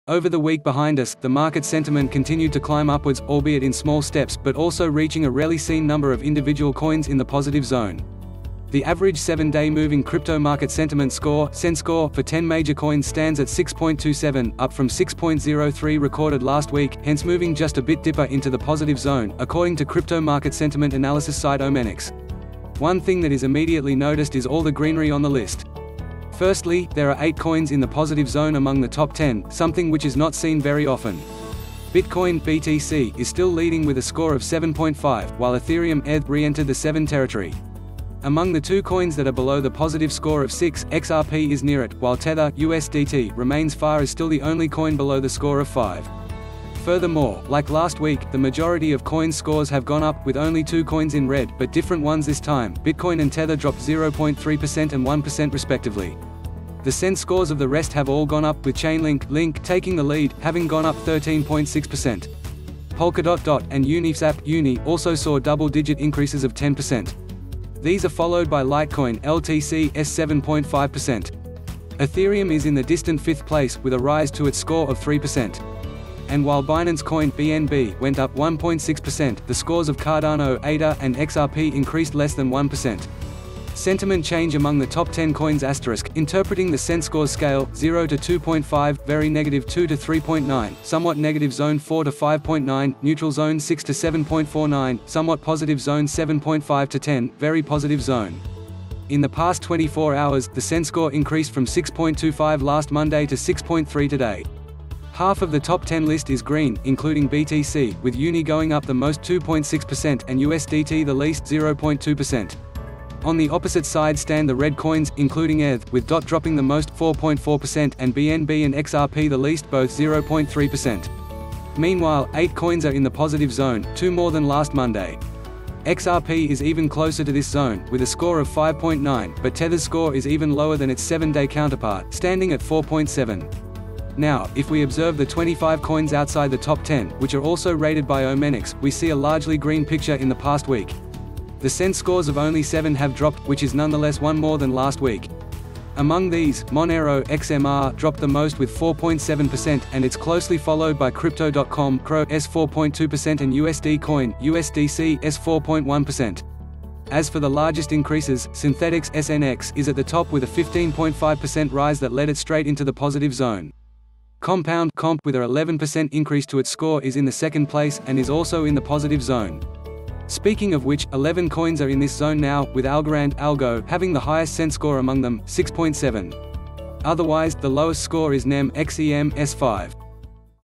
Crypto Market Sentiment Up Again ChainLink Gets the Throne
Over the week behind us, the market sentiment continued to climb upwards, albeit in small steps, but also reaching a rarely seen number of individual coins in the positive zone. The average 7-day moving crypto market sentiment score (sentscore) for ten major coins stands at 6.27, up from 6.03 recorded last week, hence moving just a bit dipper into the positive zone, according to crypto market sentiment analysis site Omenics. One thing that is immediately noticed is all the greenery on the list. Firstly, there are eight coins in the positive zone among the top 10, something which is not seen very often. Bitcoin (BTC) is still leading with a score of 7.5, while ethereum (ETH) re-entered the 7 territory. Among the two coins that are below the positive score of 6, XRP is near it, while tether (USDT) remains far as still the only coin below the score of 5. Furthermore, like last week, the majority of coins' scores have gone up, with only two coins in red, but different ones this time: bitcoin and tether dropped 0.3% and 1% respectively. The sentscores of the rest have all gone up, with chainlink (LINK) taking the lead, having gone up 13.6%. Polkadot (DOT) and uniwsap (UNI) also saw double-digit increases of 10%. These are followed by litecoin (LTC)'s 7.5%. Ethereum is in the distant fifth place, with a rise to its score of 3%. And while binance coin (BNB) went up 1.6%, the scores of cardano (ADA) and XRP increased less than 1%. Sentiment change among the top 10 coins*:Interpreting the sentscore’s scale:- 0 to 2.5: very negative- 2 to 3.9: somewhat negative zone- 4 to 5.9: neutral zone- 6 to 7.49: somewhat positive zone- 7.5 to 10: very positive zone. In the past 24 hours, the sentscore increased from 6.25 last Monday to 6.3 today. Half of the top 10 list is green, including BTC, with UNI going up the most (2.6%) and USDT the least (0.2%). On the opposite side stand the red coins, including ETH, with DOT dropping the most (4.4%) and BNB and XRP the least (both 0.3%). Meanwhile, eight coins are in the positive zone, two more than last Monday. XRP is even closer to this zone, with a score of 5.9, but tether's score is even lower than its 7-day counterpart, standing at 4.7. Now, if we observe the 25 coins outside the top 10, which are also rated by Omenics, we see a largely green picture in the past week. The sentscores of only seven have dropped, which is nonetheless one more than last week. Among these, monero (XMR) dropped the most with 4.7%, and it's closely followed by crypto.com (CRO)'s 4.2% and USD coin (USDC)'s 4.1%. As for the largest increases, synthetix (SNX) is at the top with a 15.5% rise that led it straight into the positive zone. Compound (COMP) with a 11% increase to its score is in the second place, and is also in the positive zone. Speaking of which, eleven coins are in this zone now, with algorand (ALGO) having the highest sentscore among them, 6.7. Otherwise, the lowest score is NEM (XEM)'s 5.